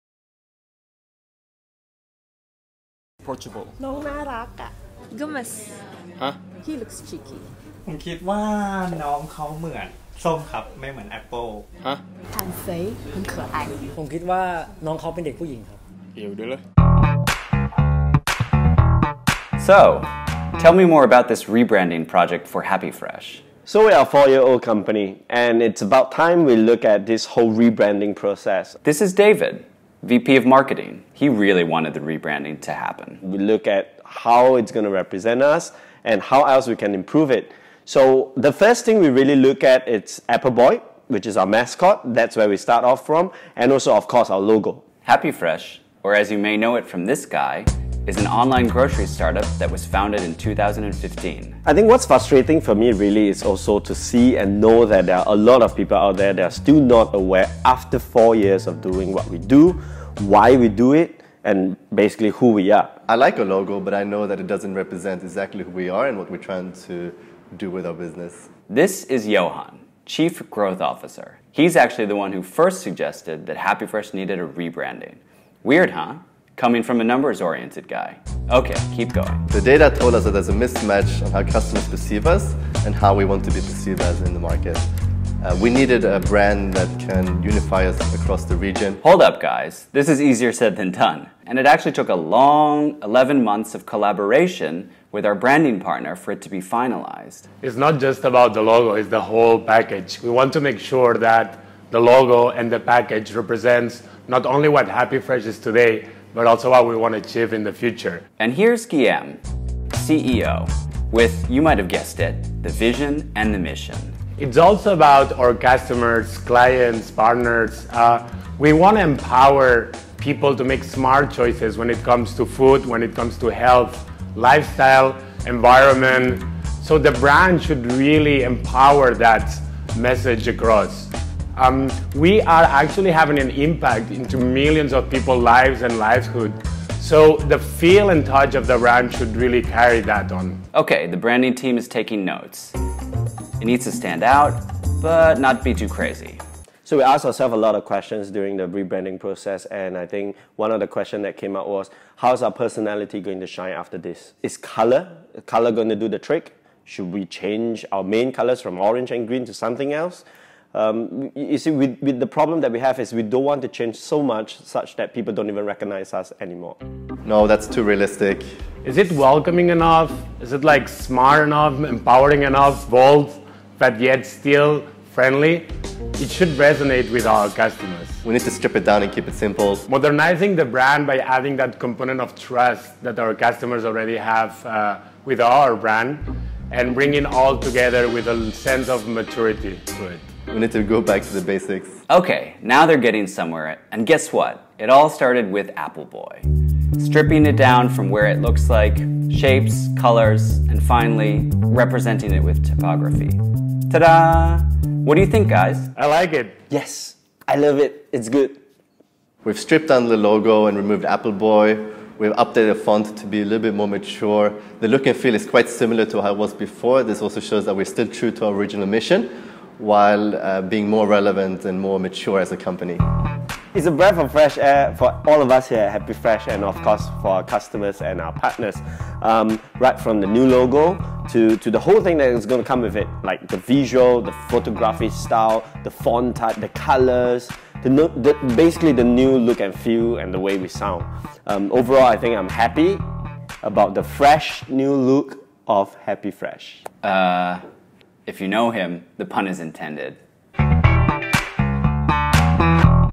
HappyFresh: Di Balik Logo Baru
Tanggal 3 Juli 2019, HappyFresh unjukkan wajah barunya ke dunia. Di antara hingar bingar semua yang serba baru, mungkin ada yang bertanya, dari mana asal semua ini? Saksikan testimoni dari kawan-kawan internal HappyFresh tentang filosofi brand dan proses transisinya di sini.